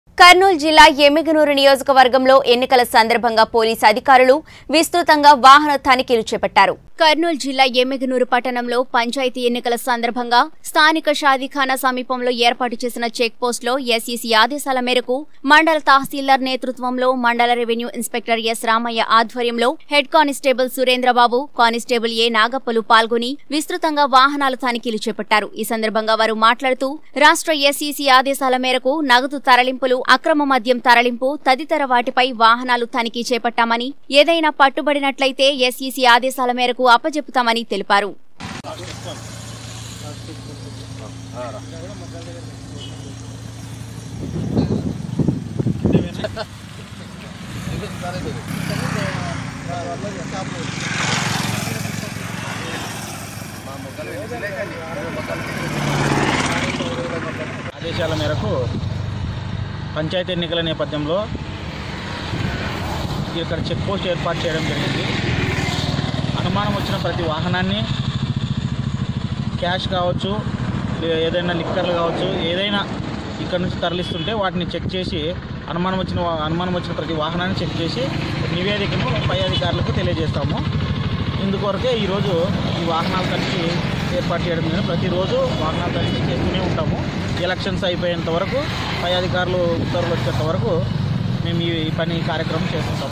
కర్నూలు జిల్లా ఎమ్మిగనూరు నియోజకవర్గంలో ఎన్నికల సందర్భంగా పోలీసు అధికారులు విస్తృత వాహన తనిఖీలు
కర్నూలు జిల్లా ఎమ్మిగనూరు నియోజకవర్గంలో ఎన్నికల సందర్భంగా పోలీసు అధికారులు విస్తృత వాహన తనిఖీలు | 19 Newstelugu